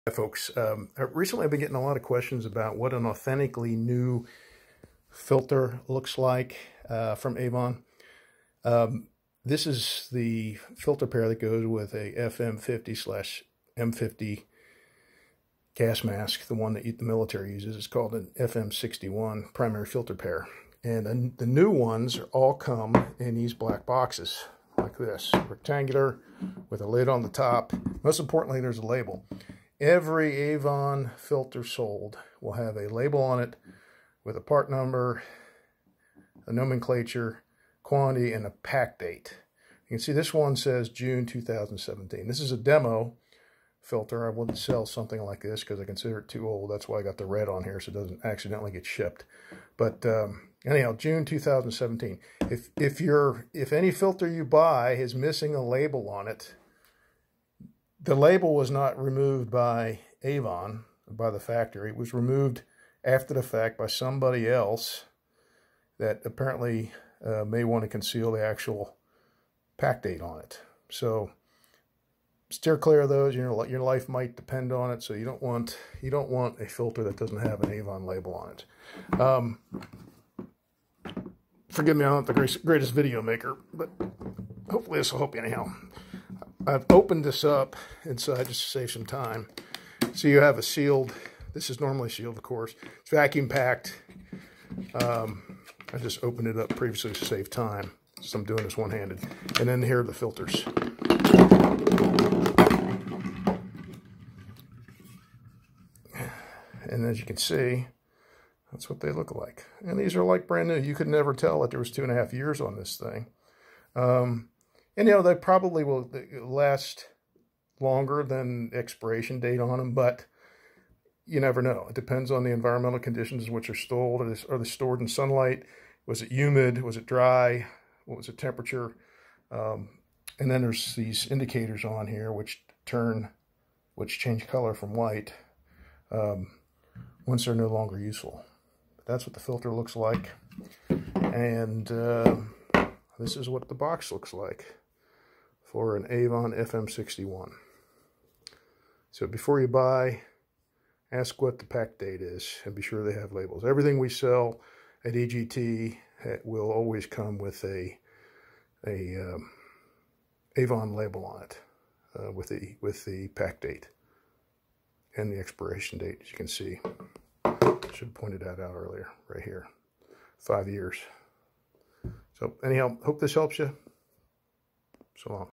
Avon FM61 Filter...Unpacking & Things To Look For
We've had some customers ask what to look for when buying a new FM61 filter, and how to avoid buying used, defective or expired filters.  Apparently there are some people selling these filters and the masks as "new" without the boxes, packaging and labeling.  This video will help you recognize filters and masks that are truly "new".  Remember, if you need your filter, you want it to work...its just not worth risking your life buying a filter with no Avon packaging or labeling.